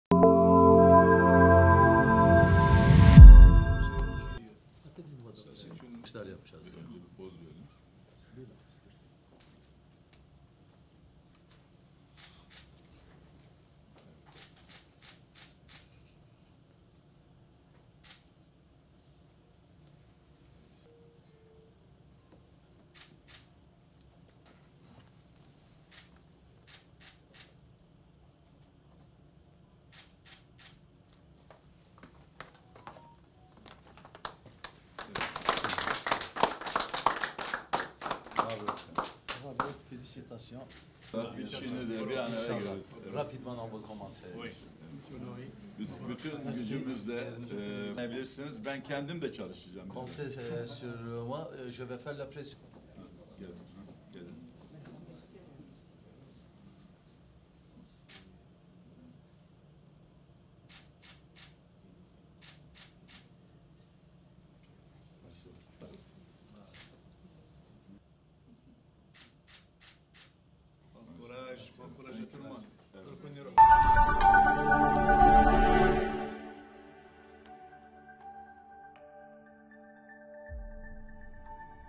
توقيع اتفاقية إطار بين سوناطراك وتوسيالي لانتاج الهيدروجين الأخضر
الجزائر(وأج)- وقع مجمع سوناطراك، اتفاقية اطار مع توسيالي، بمقر المديرية العامة لمجمع سوناطراك بالجزائر، وتهدف الاتفاقية إلى انتاج الهيدروجين الأخضر